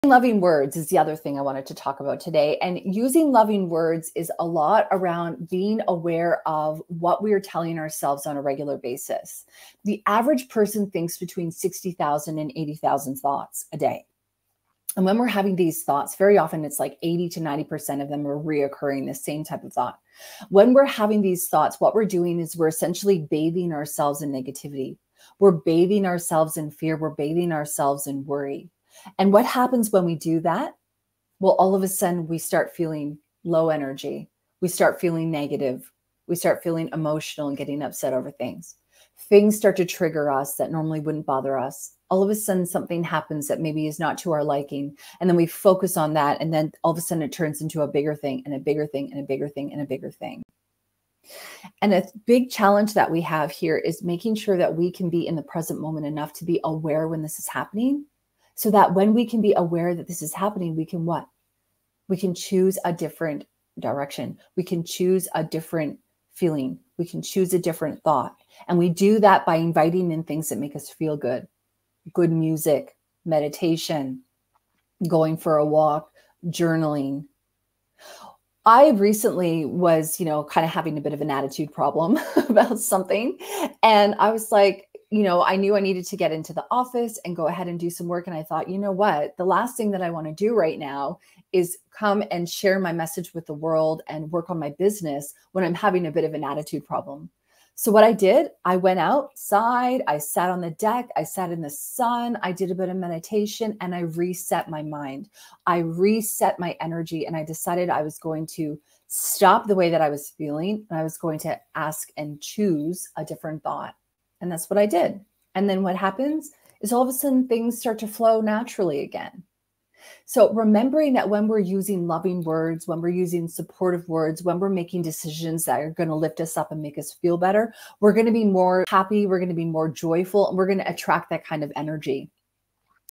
Recognising the Power of Loving Words
Lauren here, using loving words is essential in cultivating a positive mindset, and it begins with awareness of our thoughts.

When we immerse ourselves in negativity, fear, and worry, our energy and emotions suffer, triggering unnecessary stress.

A significant challenge lies in staying present to recognize these moments. However, by being aware, we empower ourselves to choose a different path—a path that brings relief, a different feeling, a different thought.

Using supportive words and uplifting decisions lifts us up and enhances our well-being.